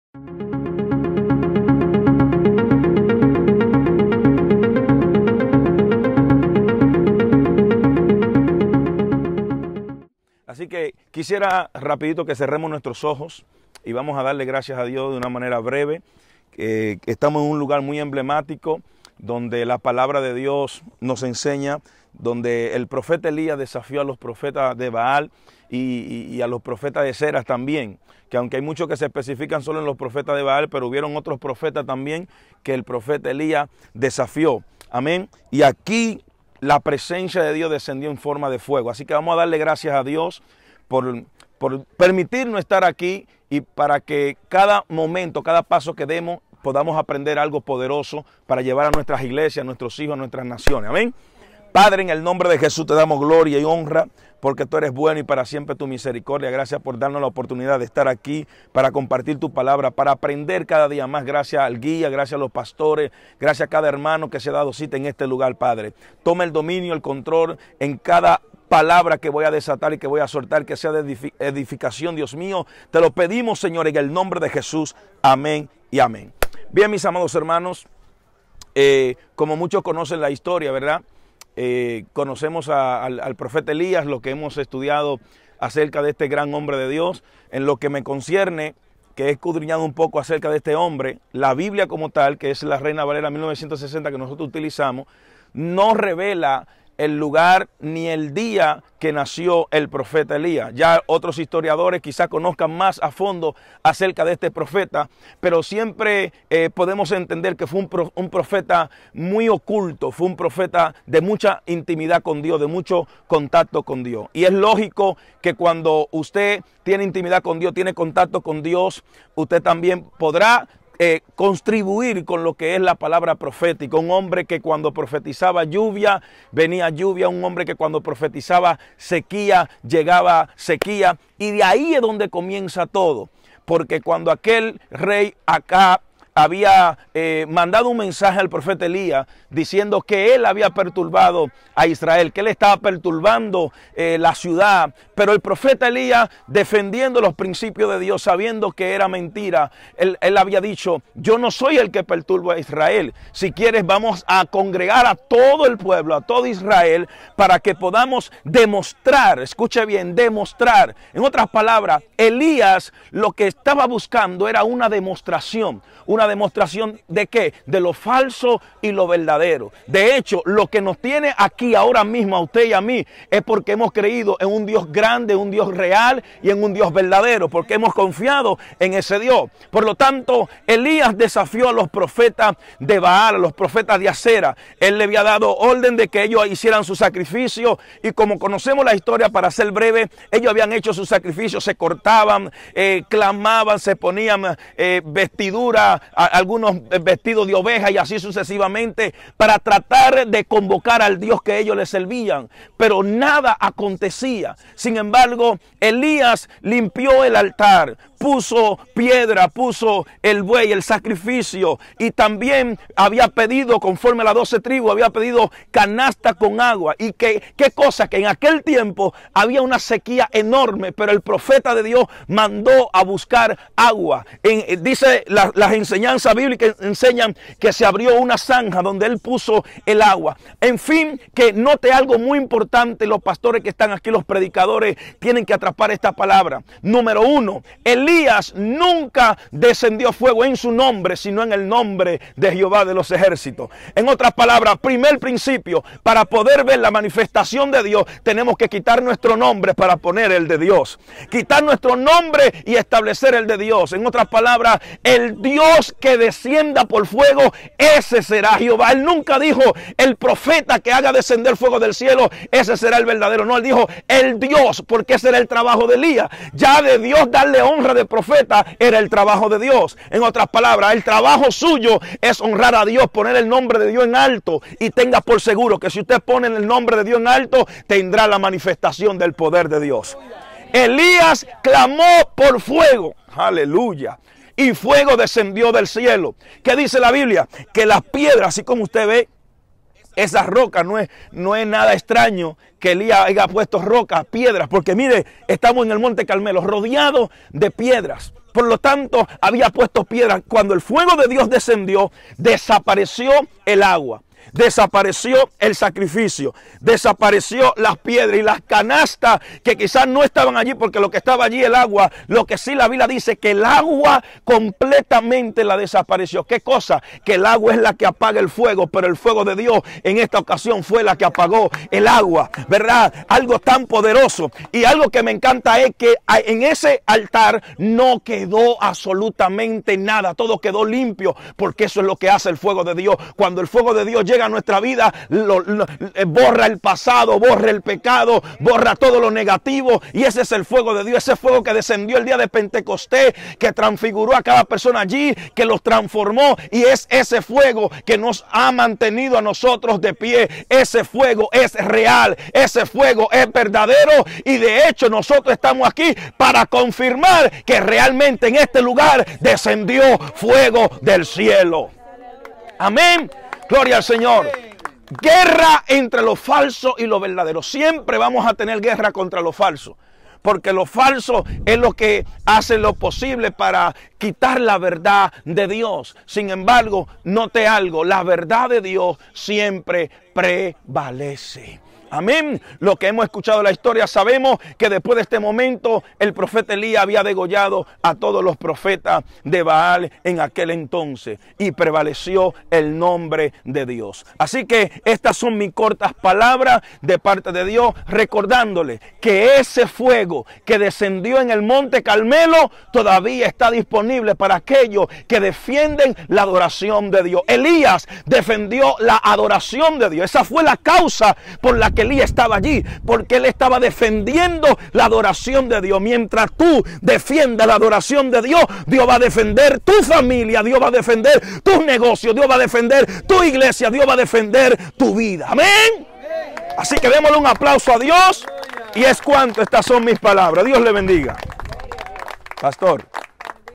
Israel Jimenez | Poderosa palabra desde el monte carmelo #405
DESDE ESTE LUGAR ELIAS CLAMO A JEHOVÁ Y ÉL LE RESPONDIO CON FUEGO.